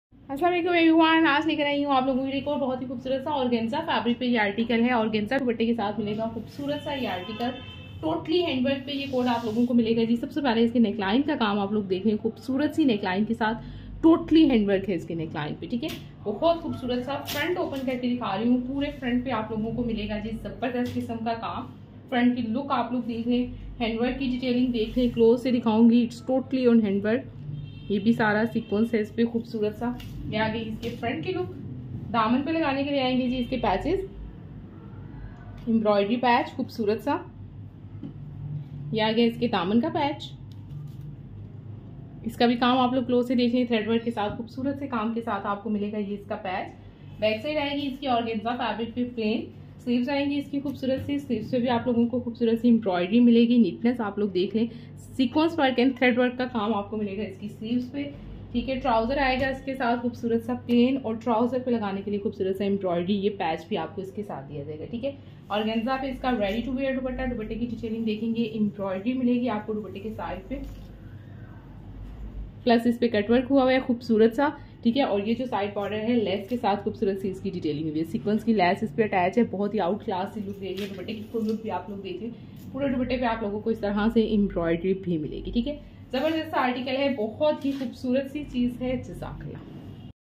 LAAM ORGANZA LUXURY FANCY SUIT
Today's Video is Very Interesting, This Vedio blongs FF Collection in Bolton Market Karachi of Fancy Party Wear Dresses, Maxi, Saree, Lawn Cotton Suits & Chikankari Suits , Pakistani Dresses you can Buy at Wholesale Price

** DC FREE **  Lawn Cotton Suit | Maxi | Frock | Saree | Party  Wear Dresses | Bolton Market Karachi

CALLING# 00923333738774